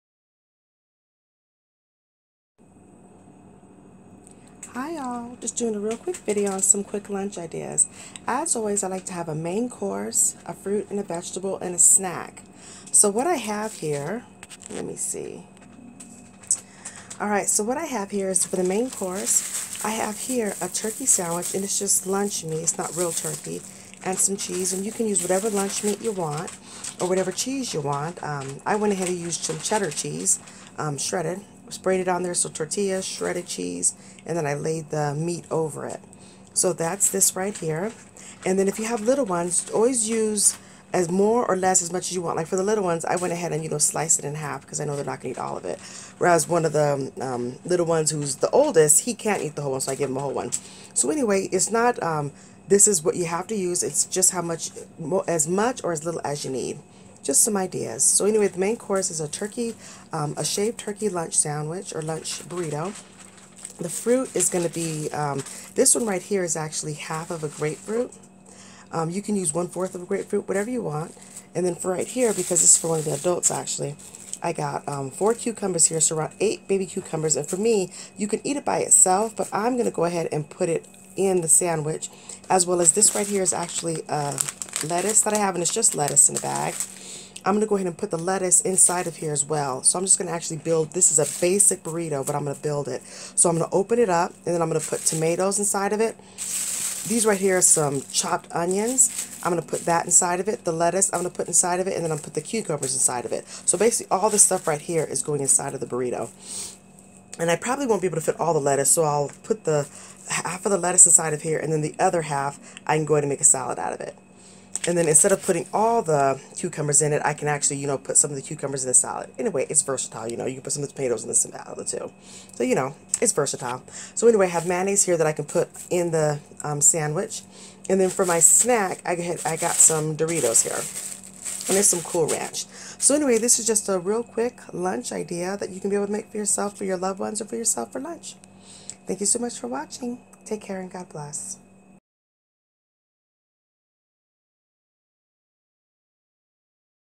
Lunch Ideas | Turkey lunch meat wrap burrito, cucumbers
Hello all,  just doing a real quick video on some quick lunch ideas.  What I am showing is a Turkey lunch meat wrap burrito, with cucumbers, grapefruit, cool ranch doritos, onions, tomatoes, and salad in a bag.